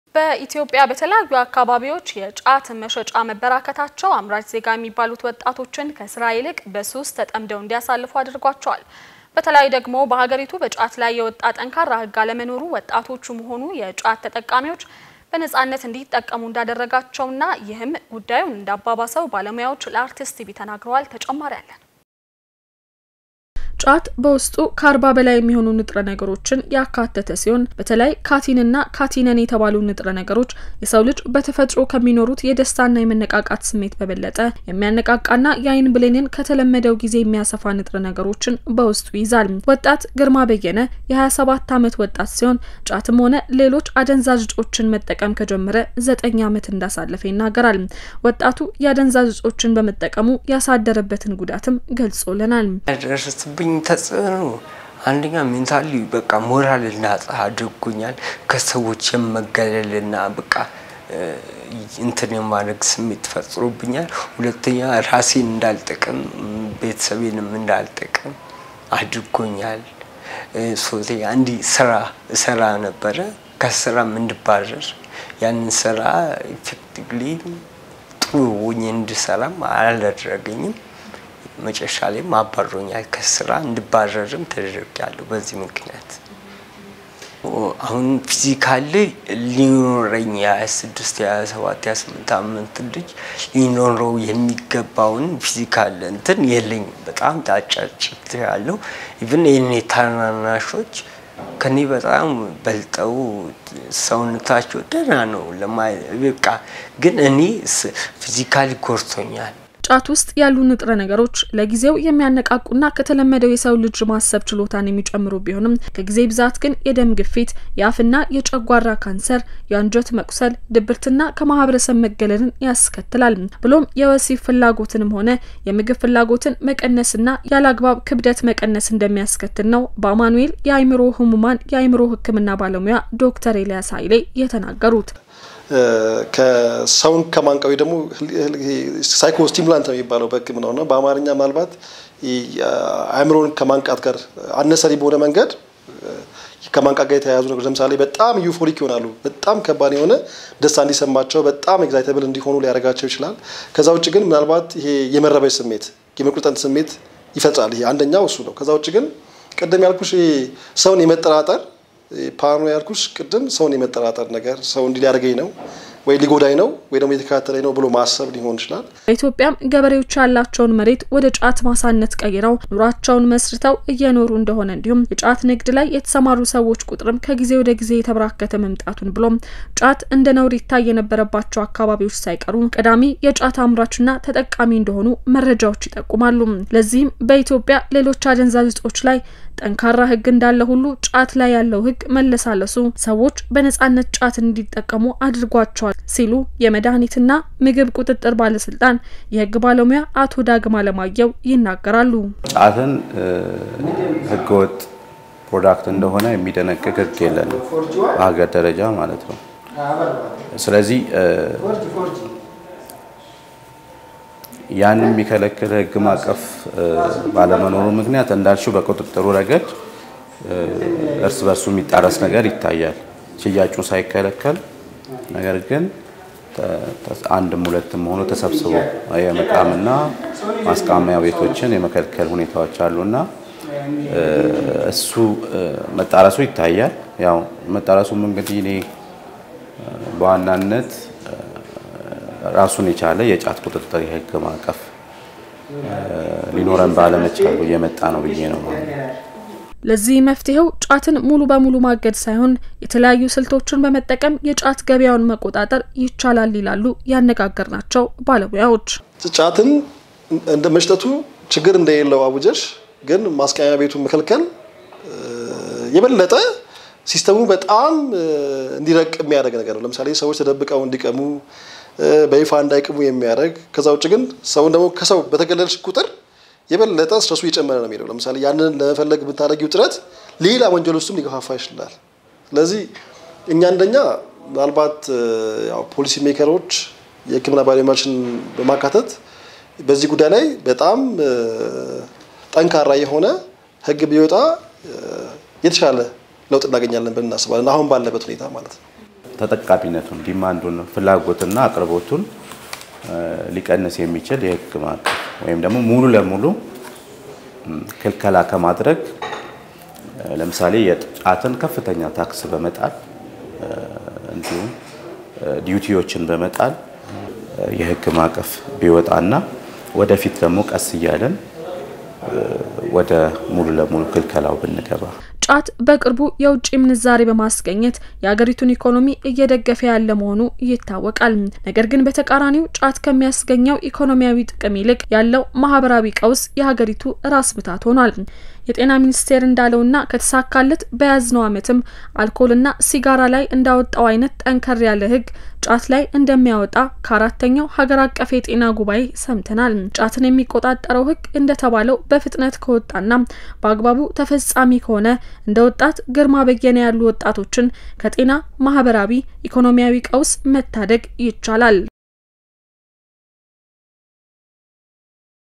የጫት መሸጫ መበራከት - News [Arts TV World]
በኢትዮጵያ በተለያዩ አካባቢዎች የጫት መሸጫ መበራከታቸዉ አምራች ዜጋ የሆነዉን ወጣት እየጎዳ ነዉ፡፡